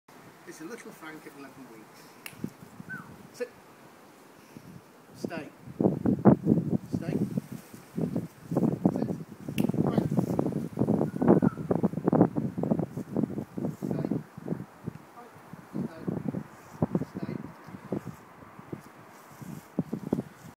Franks and distracting leaves
Quick sit and stay, but too many shiny shiny distractions.